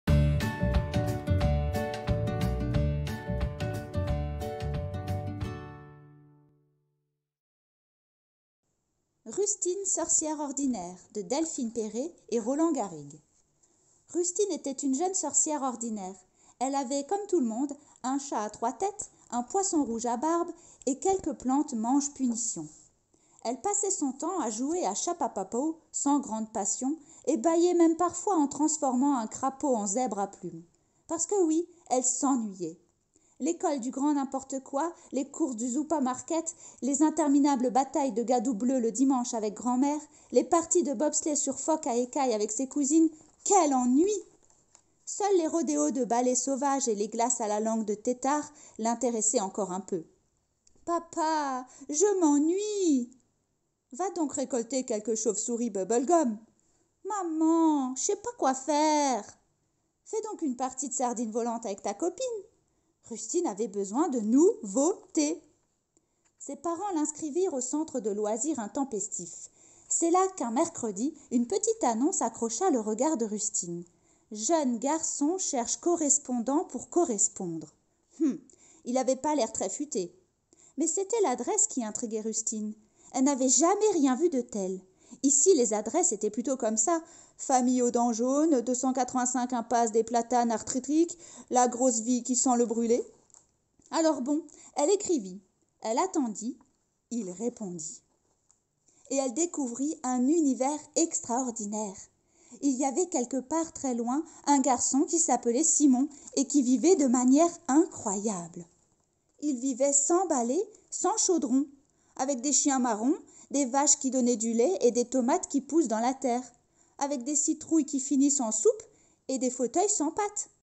[Podcast - 112 secondes de jeunesse] Rustine sorcière ordinaire
[Podcast - 112 secondes de jeunesse] Rustine sorcière ordinaire (texte de Delphine Perret, illustrations de Roland Garrigue ) , aux Editions Albin Michel Jeunesse.